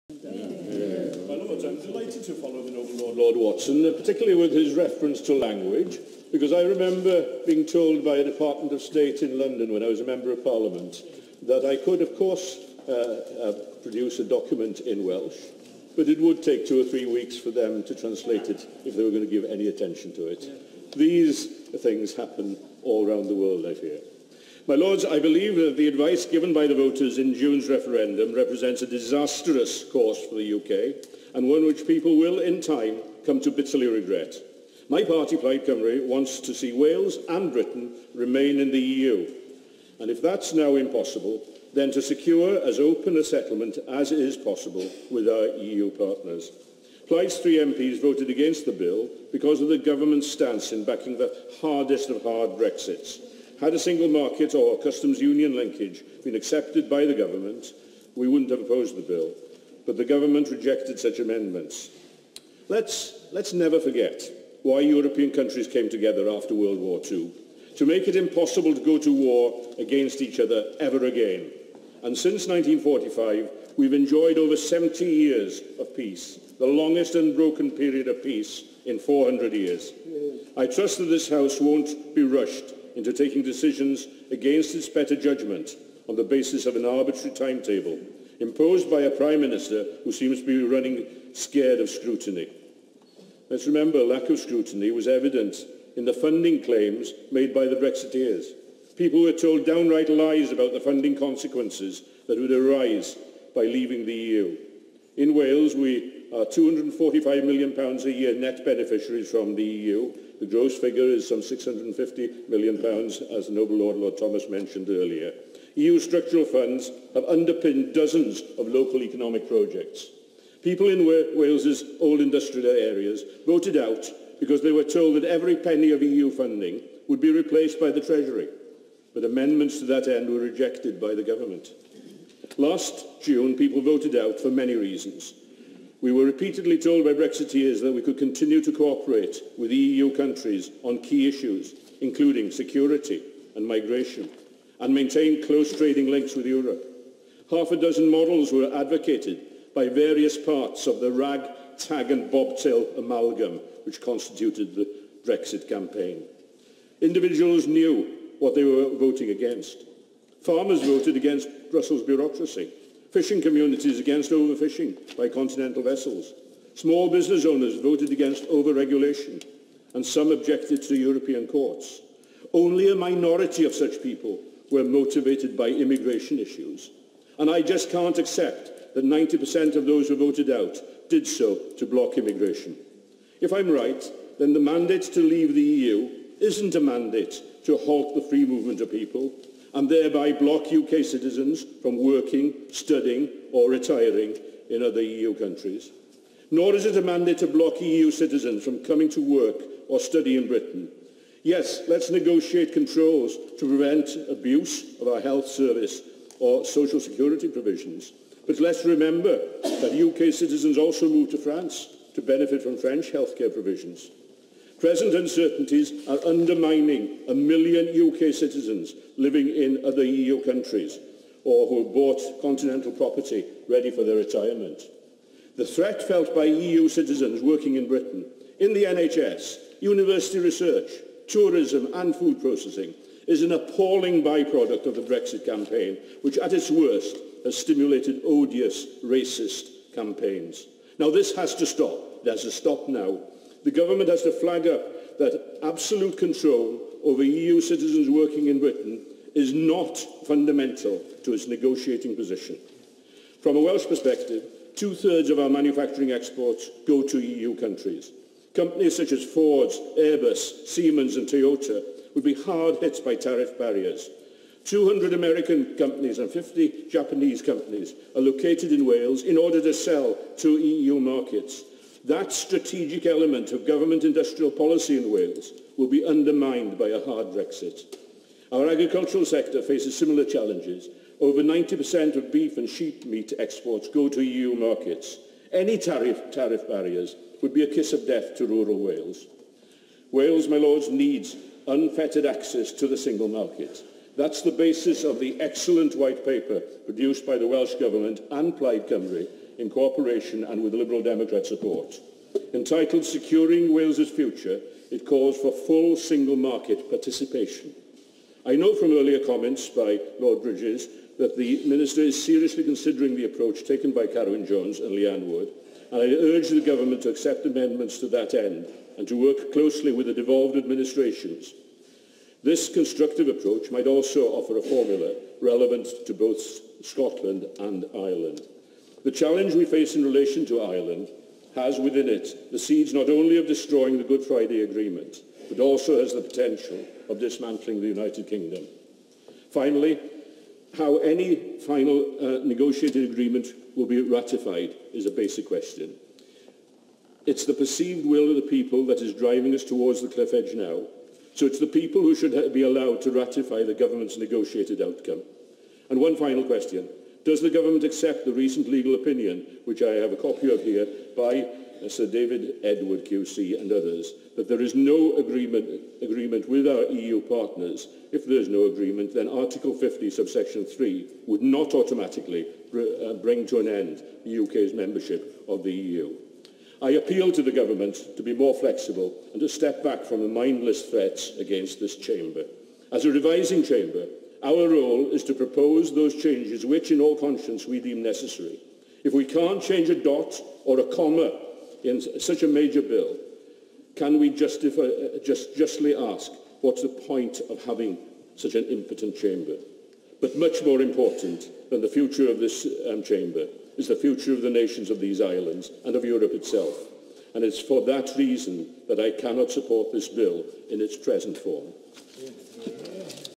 Dafydd Wigley Brexit Speech in House of Lords
Mewn araith yn Nhŷ'r Arglwyddi dywedodd Dafydd Wigley bod angen sicrhau bargen gyda'r UE wnaiff warchod economi Cymru ac bod dybryd angen rhoi terfyn ar y gwaddol o gasineb a hiliaeth y refferendwm / In a speech in the House of Lords Dafydd Wigley said that it's essential to secure a deal with the EU which protects the Welsh economy and that the referendum's legacy of hate and racism must be stopped now.